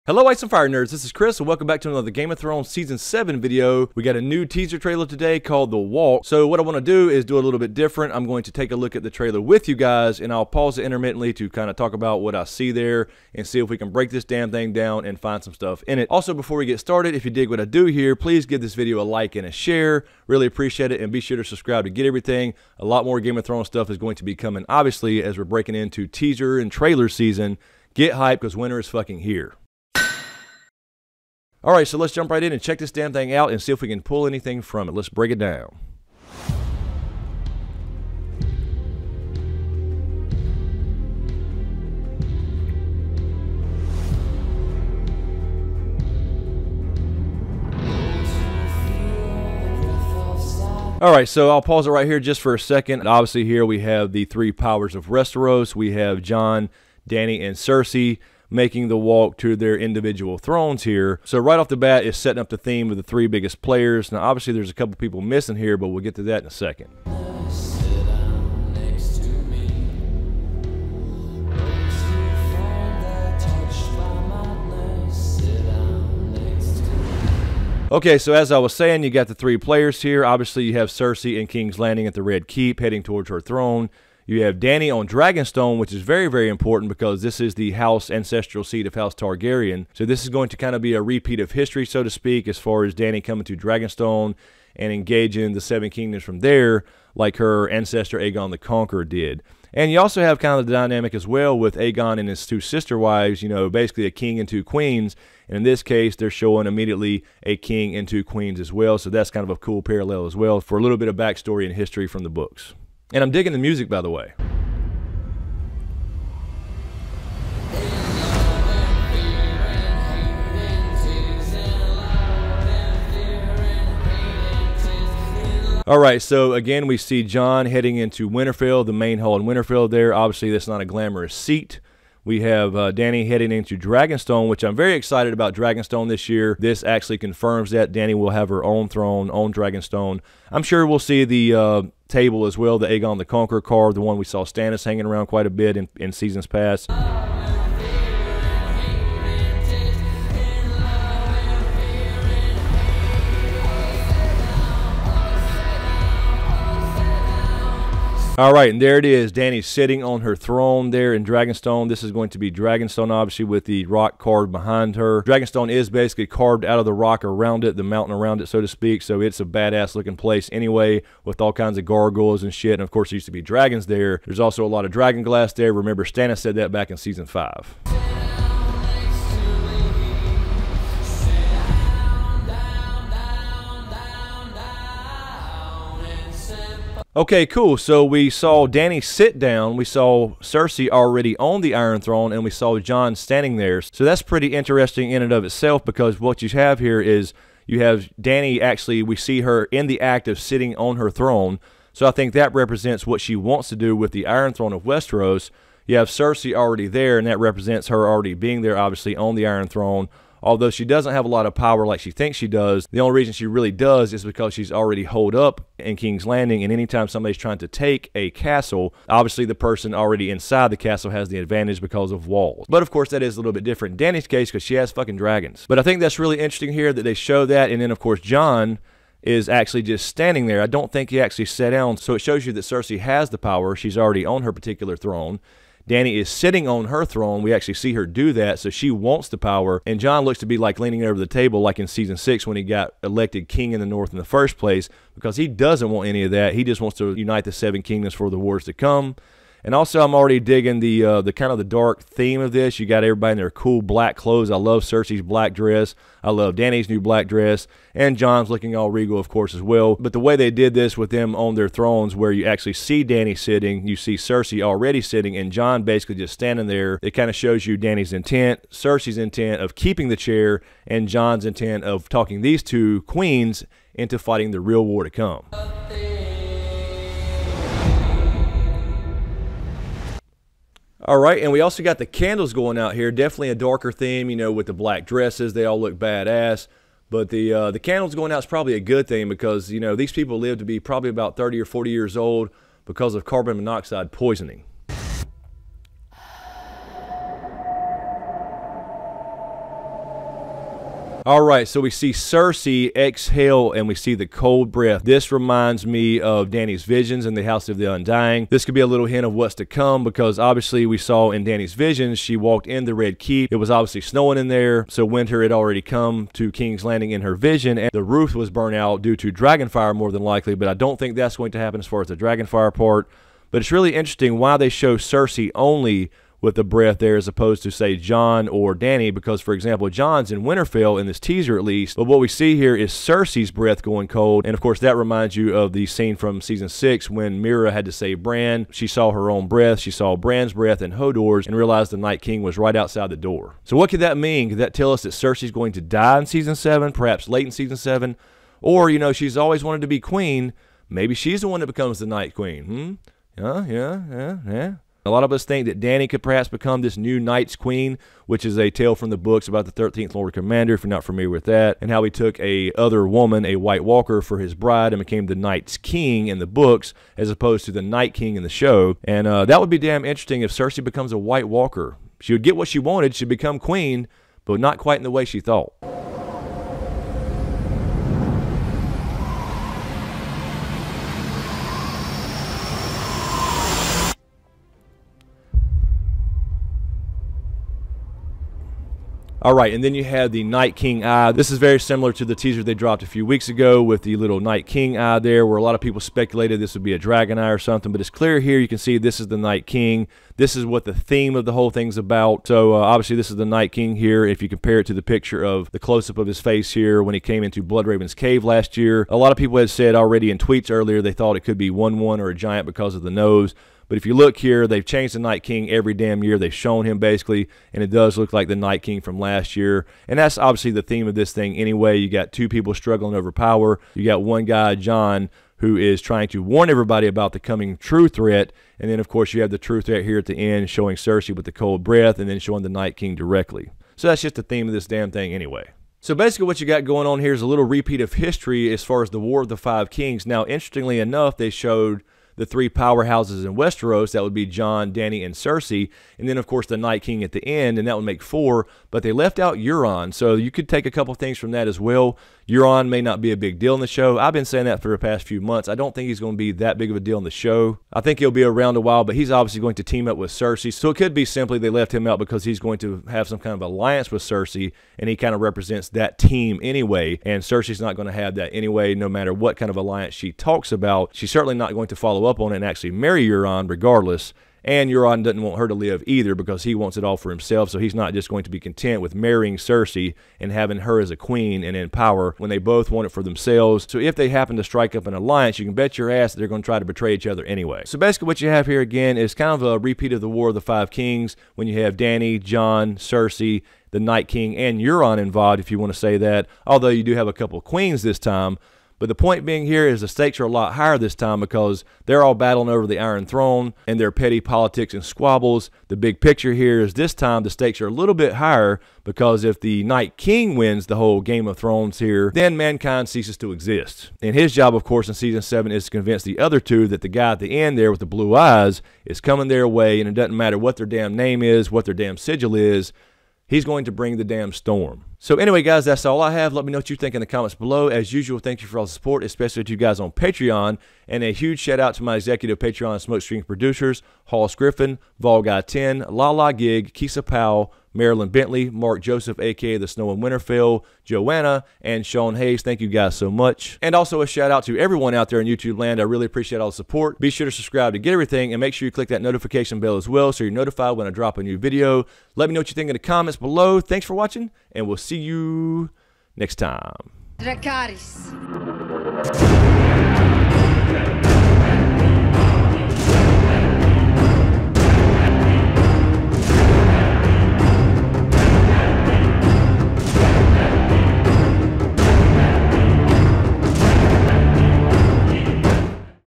Game of Thrones Season 7: Long Walk - Official (HBO) Promo Breakdown
Game of Thrones Season 7: Long Walk - Official Promo Breakdown

**Potential Spoilers**

Game of Thrones season 7 Teaser Trailer, Long Walk - Official Promo Breakdown. Jon, Dany and Cersei take their respective thrones, as Winter and the Night King arrive in Westeros. Game of Thrones Season 7 is coming.

Send Stuff:
Lochmoor Productions

Hoss Griffin, VolGuy10, Lala Gig , Kissa Powell, Marc Joseph aka The Snow In Winterfell, Marylin Bentley, JoAnna, Sean Hayes